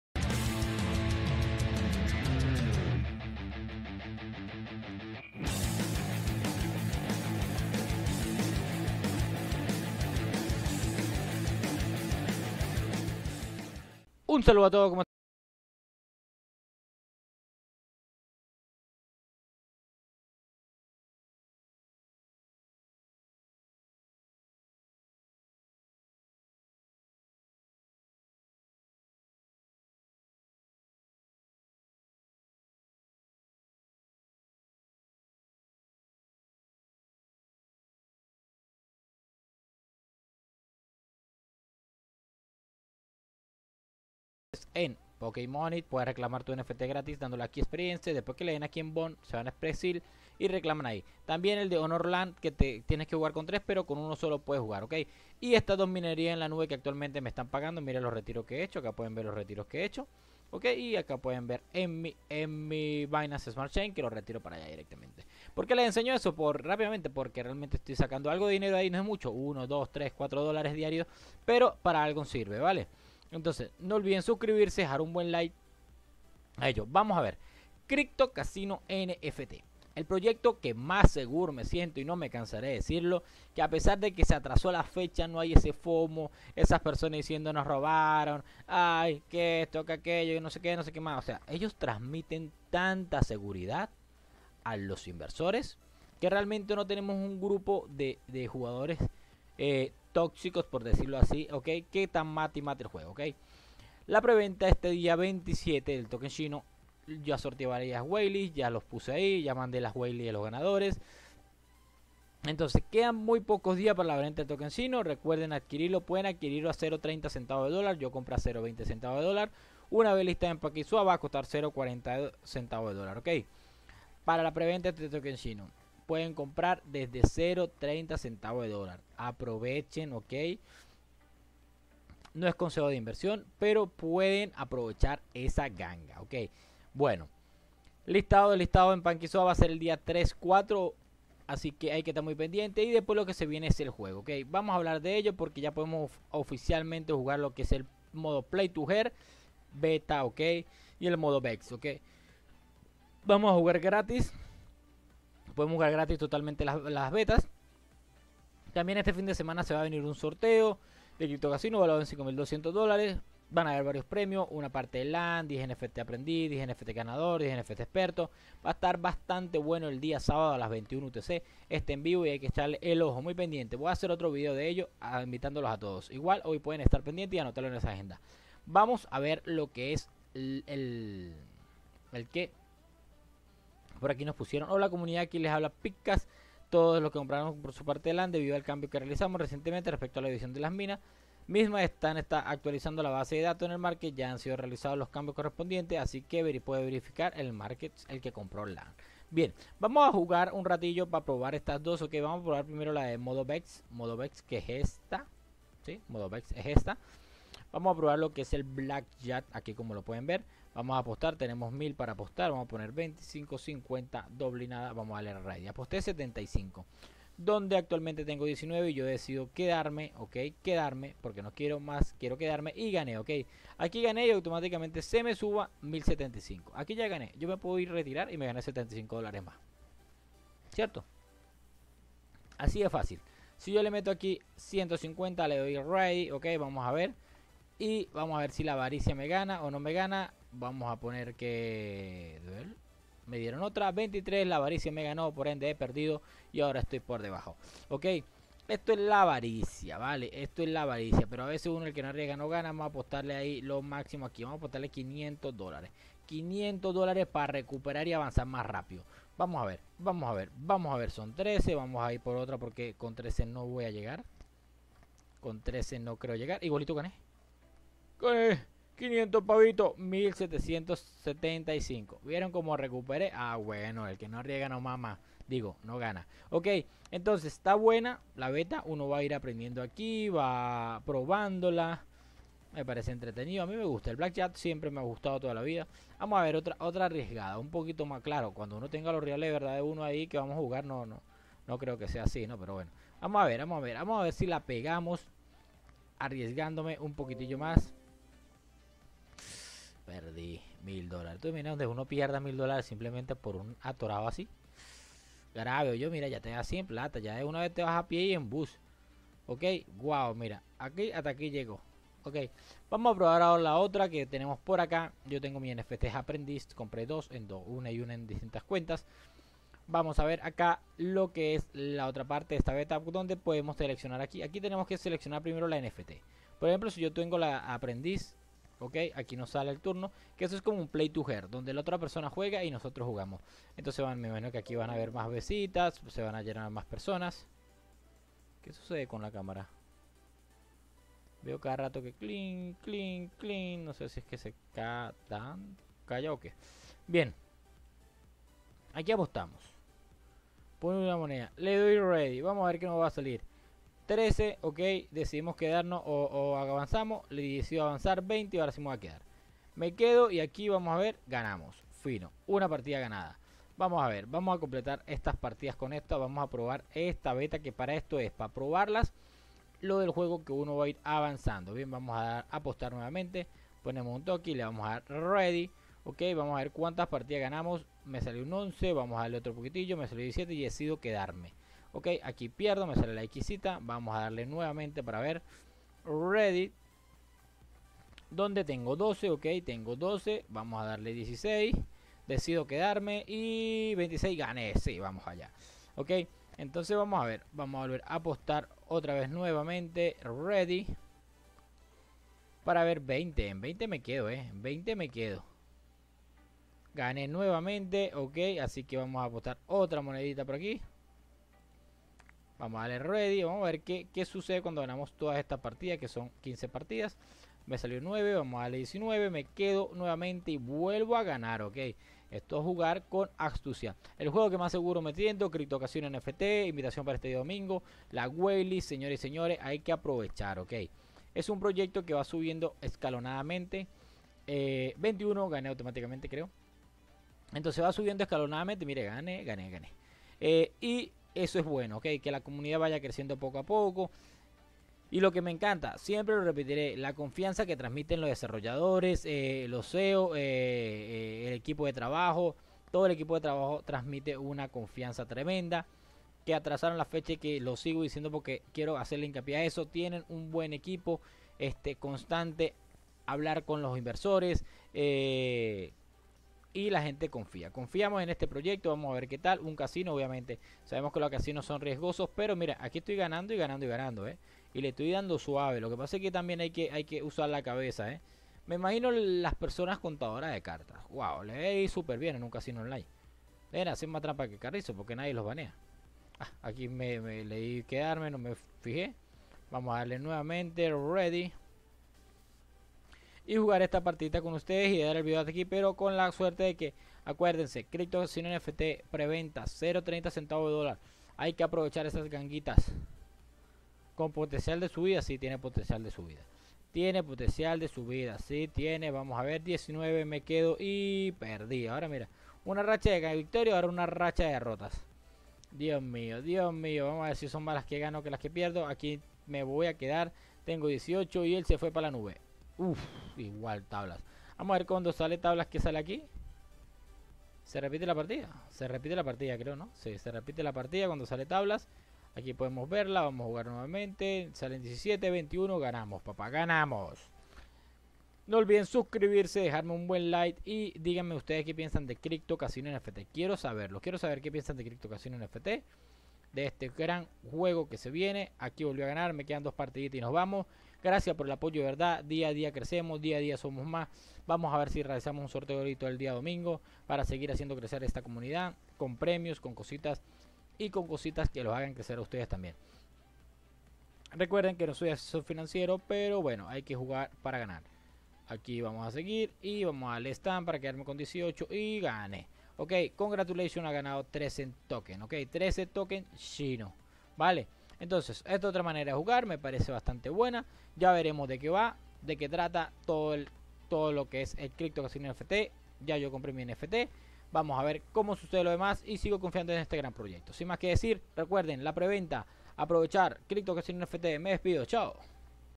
CRYPTO CASINO NFT 🃏TODO LISTO PARA LA PRE VENTA🤑PROBANDO LA BETA🔥🤑
pagina web

📍Información importante sobre la PREVENTA II:

♦️Cantidad: 500.000
♦️Soft Cap: 100.000 tokens.
♦️Liquidez: 70%.
♦️Moneda de venta: USDT/BUSD.
♦️Mínimo de compra: 30.
♦️Máximo de compra: 1200.
🗓️Fecha: 27/03/22,  desde las 12 PM UTC hasta 1 AM UTC del 28/03.
📍Por plataforma propia.

👉🏻Vesting: 40% TGE, resto 20% cada 20 días. (Recupero total 60 días).

📌Listamiento: 03/04/22.

‼️Con los cynos adquiridos en preventa, podrás adquirir NFT y LANDS a un precio promocional.

HORARIOS SEGÚN PAÍS:

🇦🇷 09:00 hs
🇪🇸 13:00 hs
🇲🇽 07:00 hs
🇻🇪 08:00 hs
🇨🇴 07:00 hs

WHITEPAPER

canal pasqualefz